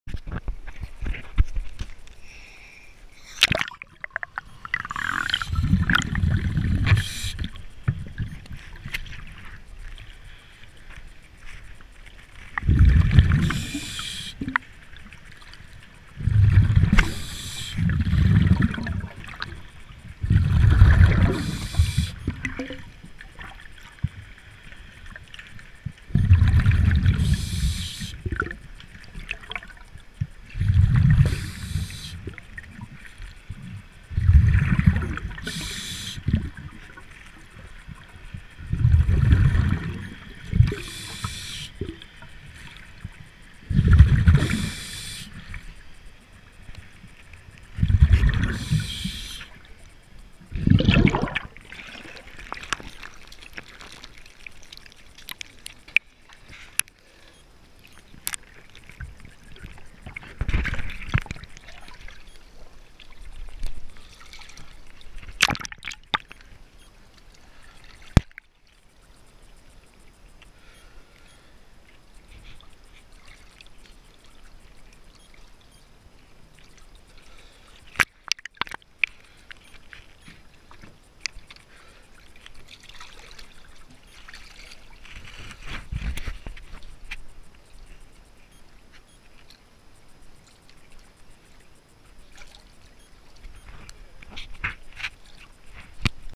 C Drive 11:20:22
Peninsula Dive Services cleaning for Bruce Nobles boat cleaning.